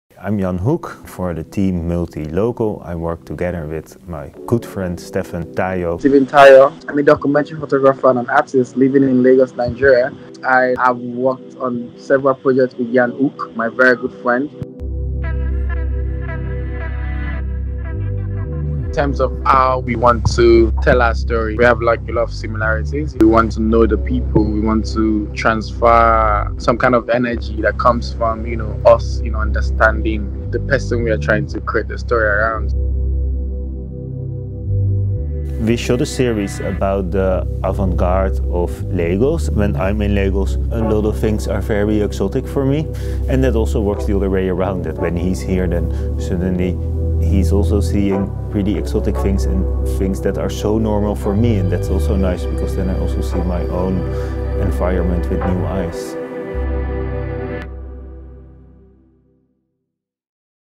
Heimtextil 2020 – Behind the Scenes with Jan Hoek and Stephen Tayo
Despite being based on different continents, the distance between Stephen Tayo and Jan Hoek only strengthens their creative work together on Multi-Local and beyond. 🌍